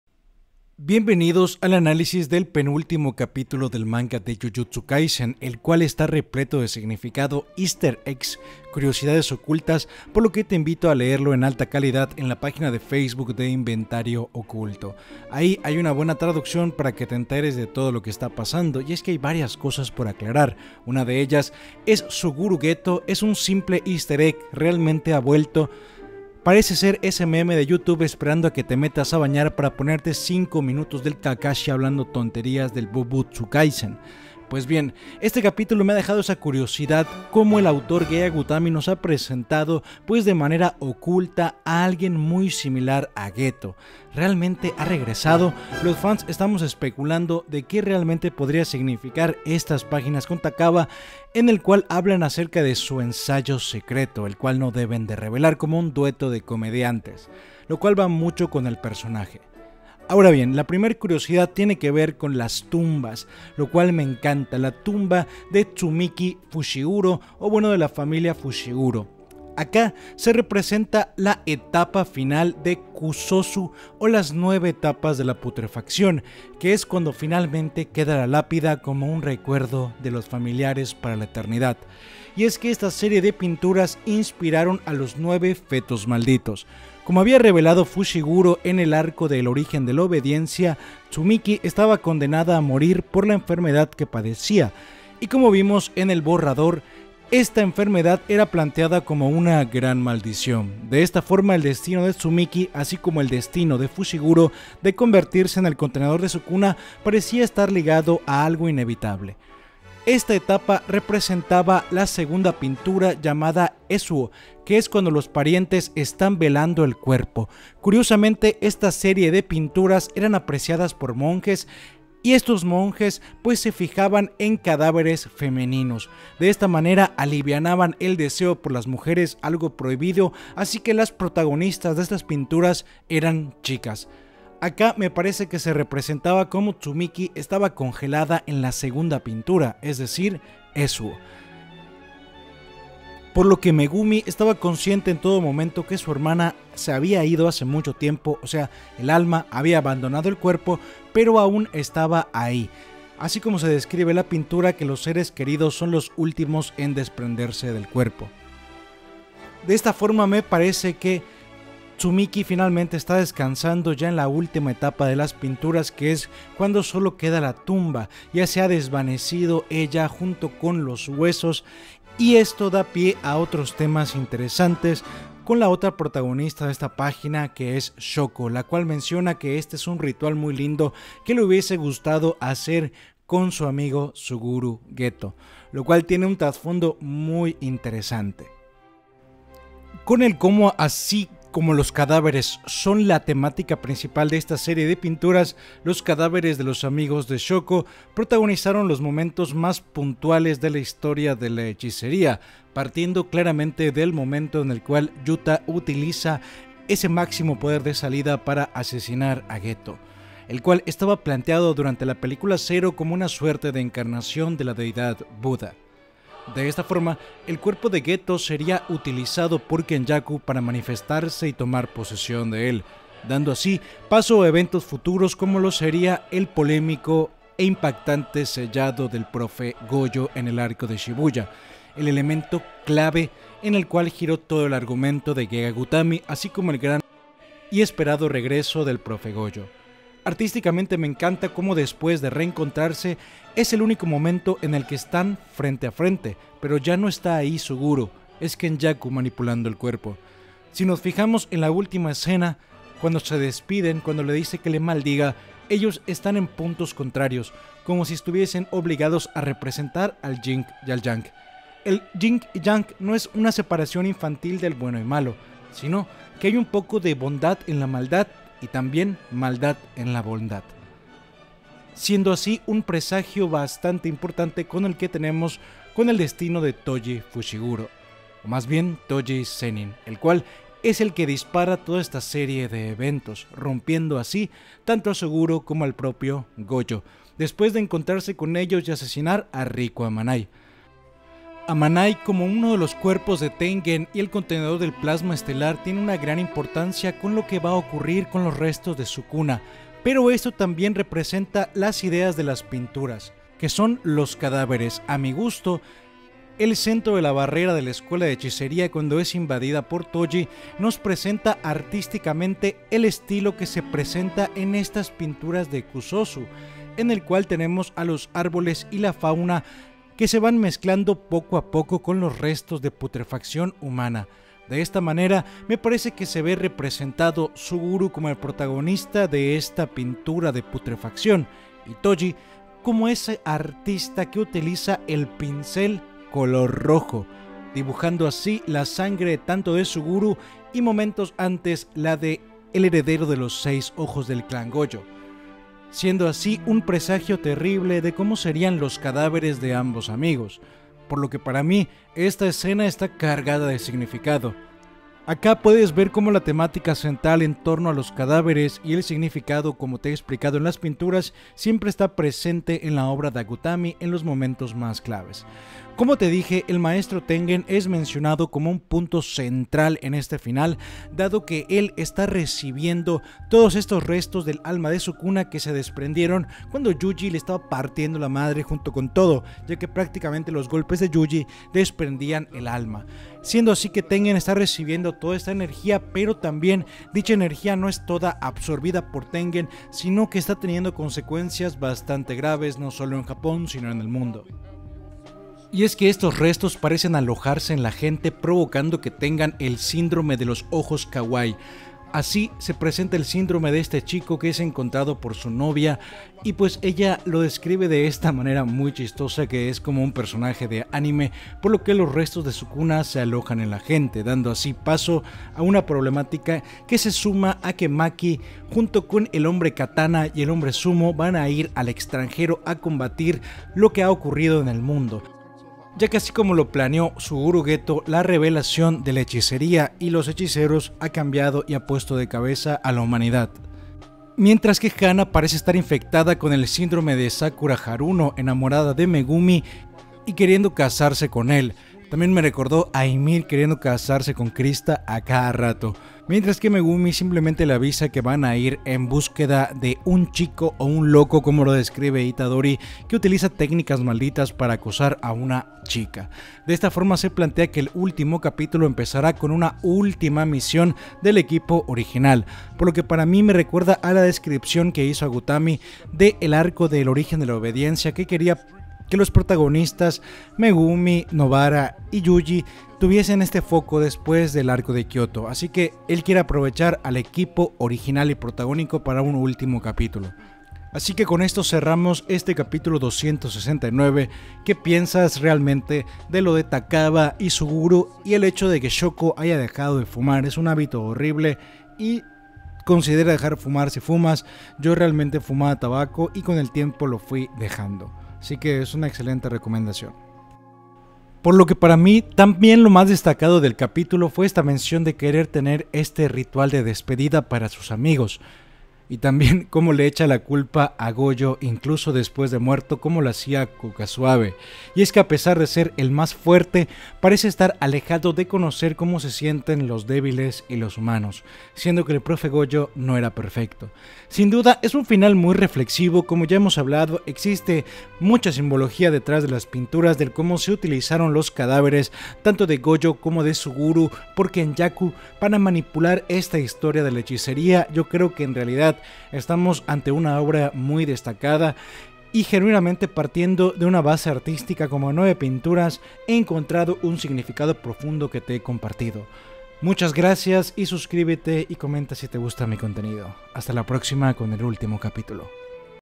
¿Regresó GETO? ¿El FINAL MÁS RANCIO de LA HISTORIA? Jujutsu Kaisen 270
JujutsuKaisen jujutsuKaisen270 geto suguru En este video hablemos del inesperado regreso de Geto entre otros elementos del final de Gege Akutami para Jujutsu Kaisen y lo que plantea será un ultimo capitulo enfocado en el trio protagónico principal.